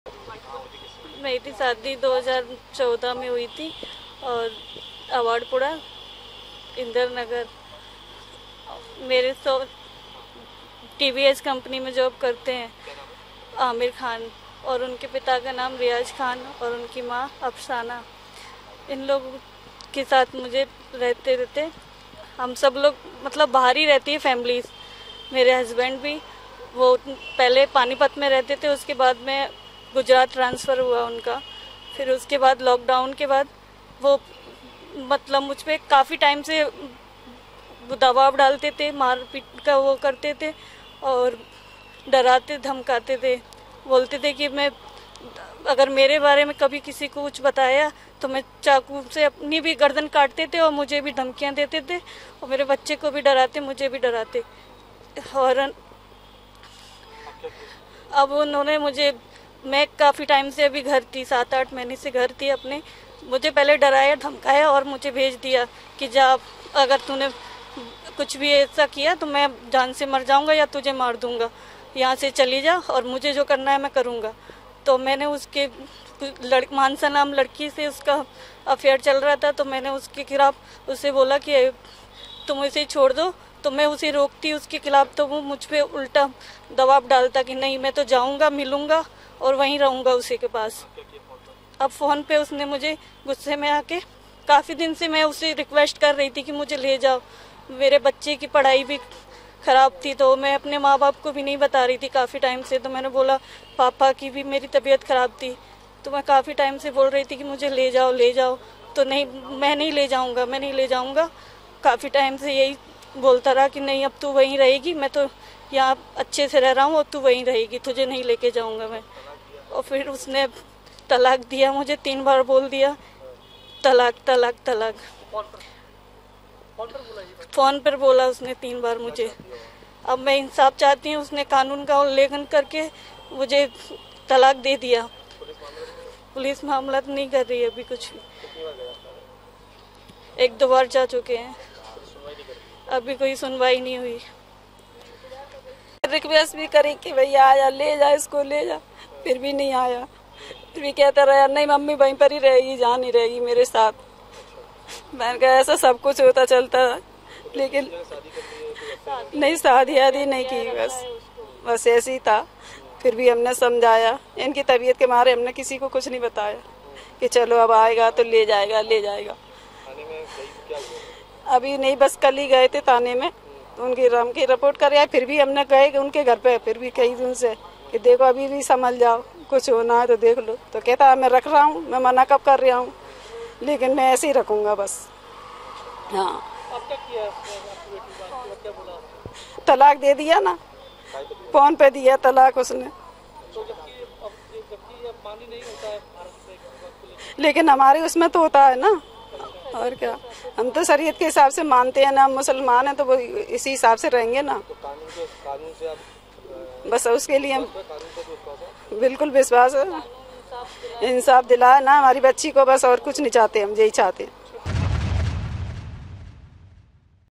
Gwalior से हैरतंगेज मामला सामने आया जहां शौहर Aamir Khan ने पत्नी को फोन पर दिया तीन तलाक
ग्वालियर में एक ट्रिपल तलाक का मामला सामने आया है। इंजीनियर  शौहर आमिर खान ने गुजरात से फोन लगाकर ग्वालियर में बैठी अपनी बीबी को तीन तलाक दे दिया। शौहर ने 9 साल की शादी महज़ 9 सेकेंड में तोड़ दी, तो वही परेशान पत्नी ने ग्वालियर पुलिस से शिकायत की है। पुलिस ने शिकायत के बाद मामले में कार्रवाई का भरोसा दिलाया है।